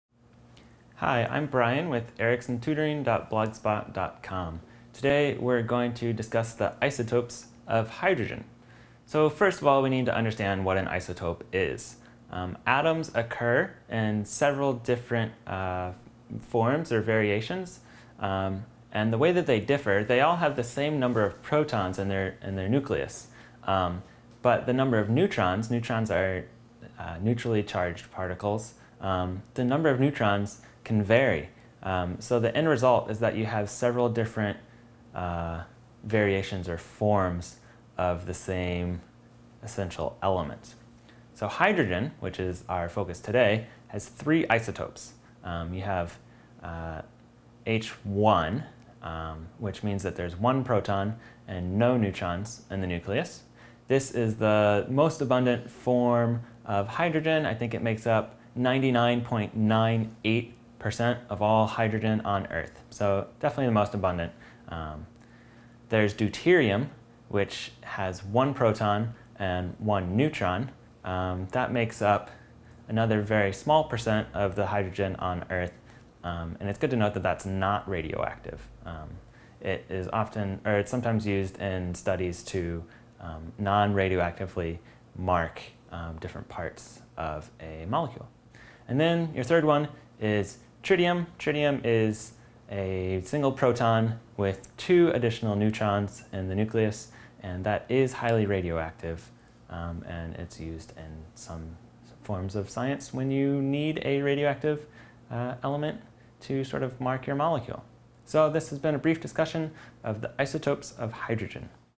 Biology & Organic Chemistry : What Are the Isotopes of Hydrogen?
Hydrogen has three isotopes, which are H1, deuterium and tritium. Learn about the makeup of the isotopes of hydrogen with help from a science teacher and field biologist in this free video on chemistry.